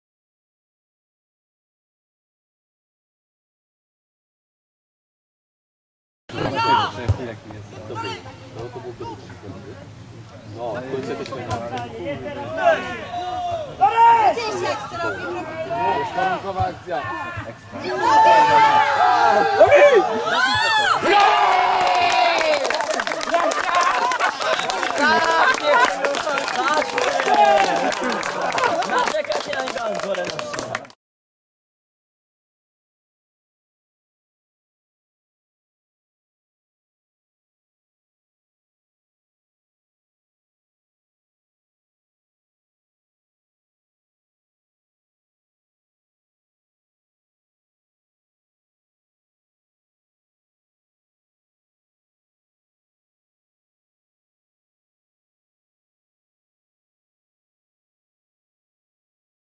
Adrian Haszczyc (1:1) 30' 3 kolejka B: Płomień II - Uran Łukowica
Bramka na 1:1 w meczu 3 kolejki B klasy: Płomień II - Uran. Strzelcem gola Adrian Haszczyc.